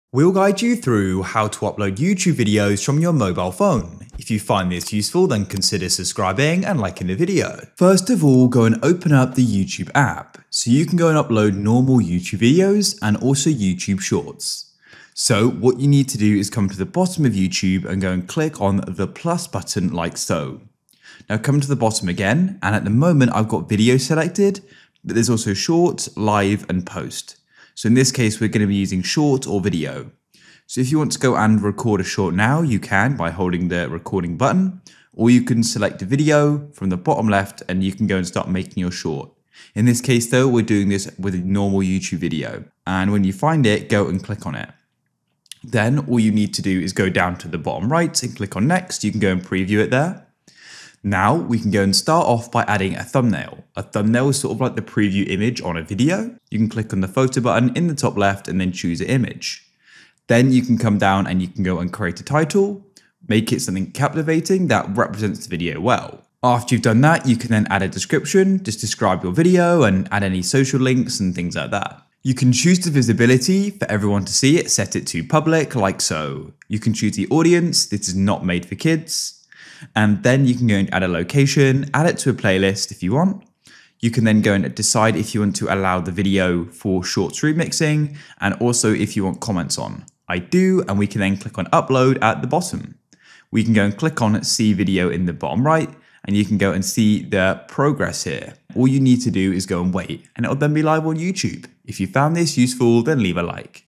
How To Upload YouTube Videos From Phone - Full Guide
Learn how to upload youtube videos from phone in this video.

GuideRealm